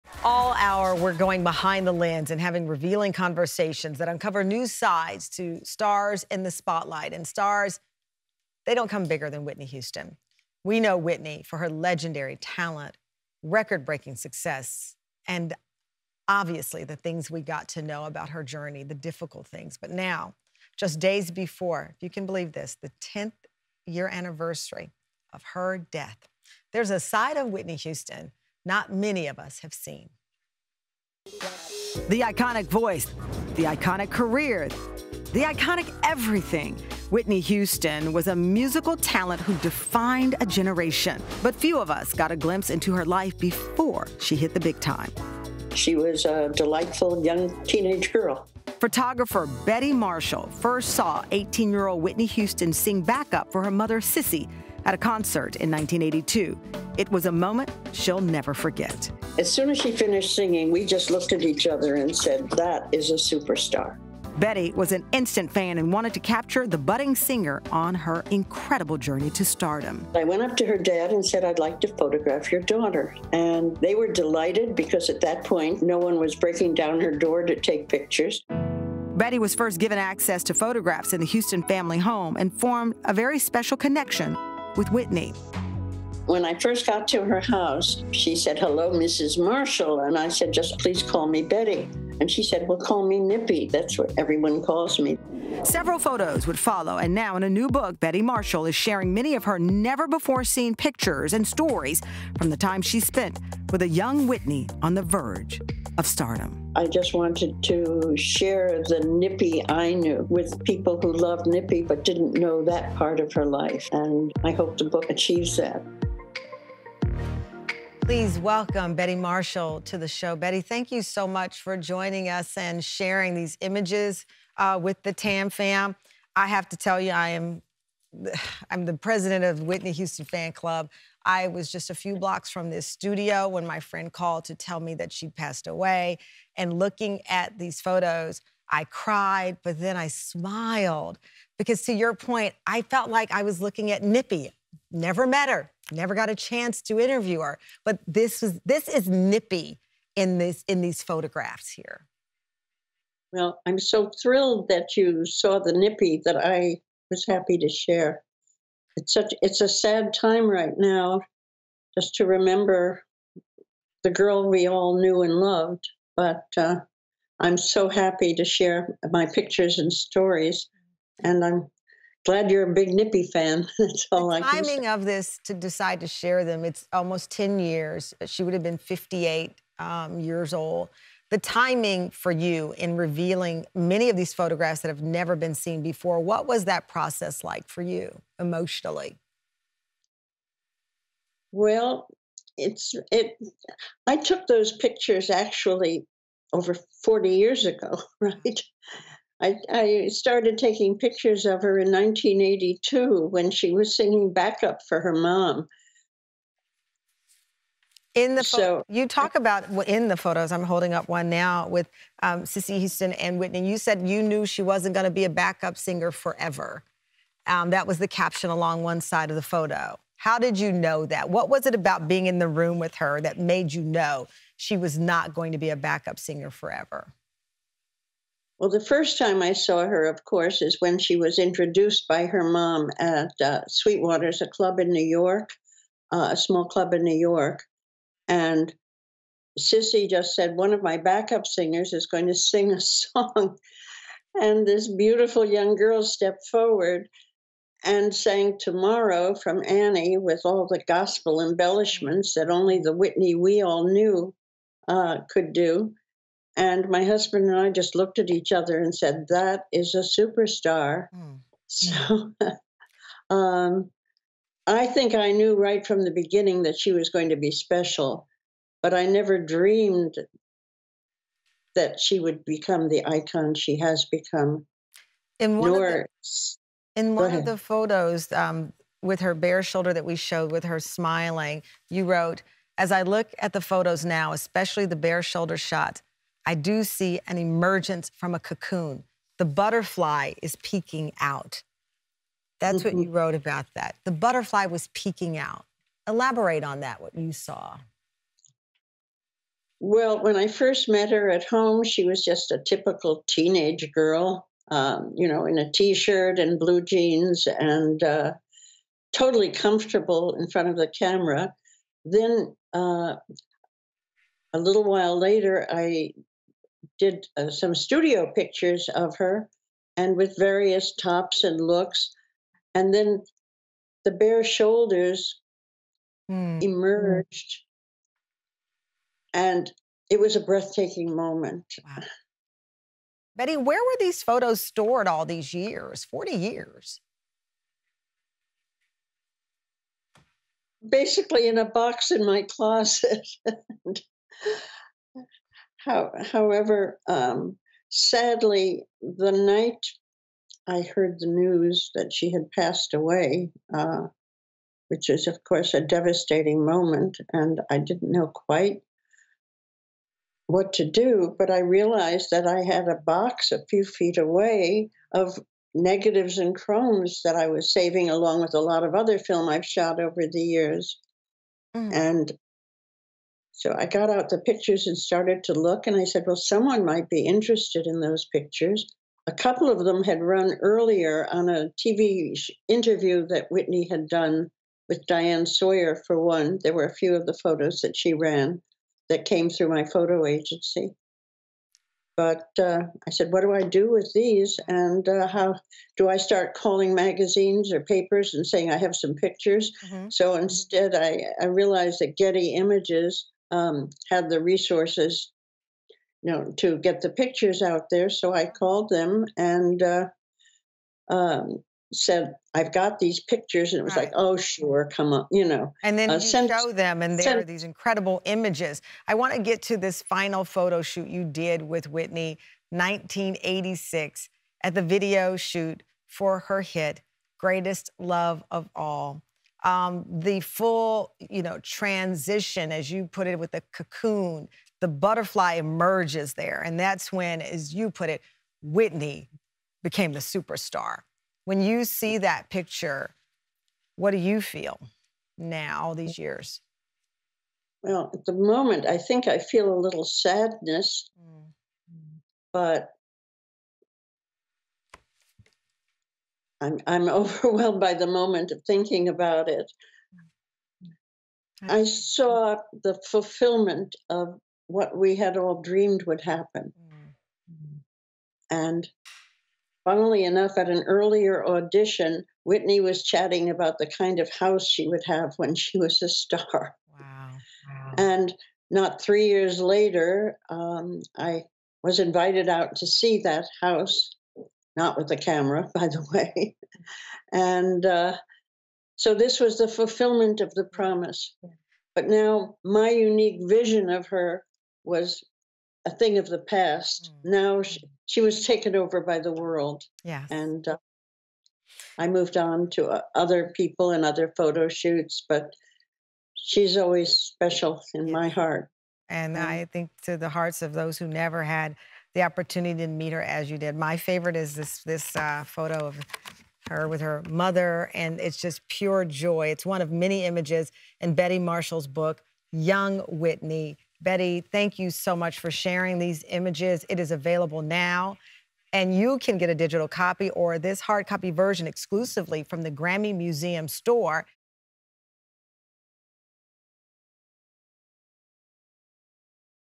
She Photographed a Young Whitney Houston Before the Fame
Bette Marshall is a highly regarded photographer whose work has appeared in The New York Times, Vogue, People, Rolling Stone, and more. When Whitney Houston was a budding star, Bette photographed her on many occasions. In her new book, “Young Whitney: Stories & Photographs,” she shows never-before-seen shots of the iconic singer after meeting her at just 18 years old. Bette also talks to Tamron about seeing Whitney’s star power well before the world knew who she was.